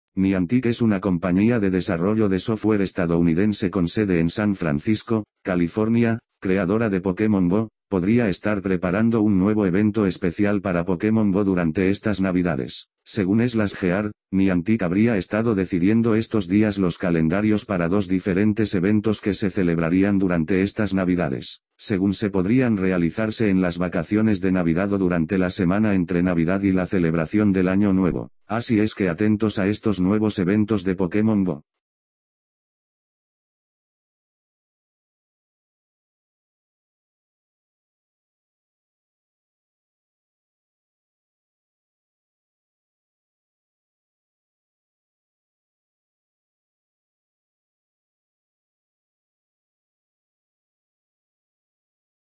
Pokemon Go Evento De Navidad - Confirmado
Niantic es una compañía de desarrollo de software estadounidense con sede en San Francisco, California, creadora de Pokemon GO,  podría estar preparando un nuevo evento especial para Pokémon GO durante estas navidades.

Según Slash Gear, Niantic habría estado decidiendo estos días los calendarios para dos diferentes eventos que se celebrarían durante estas navidades: según se podrían realizarse en las vacaciones de Navidad o durante la semana entre Navidad y la celebración del año nuevo.

Asi es que atentos a estos nuevos eventos de Pokemon Go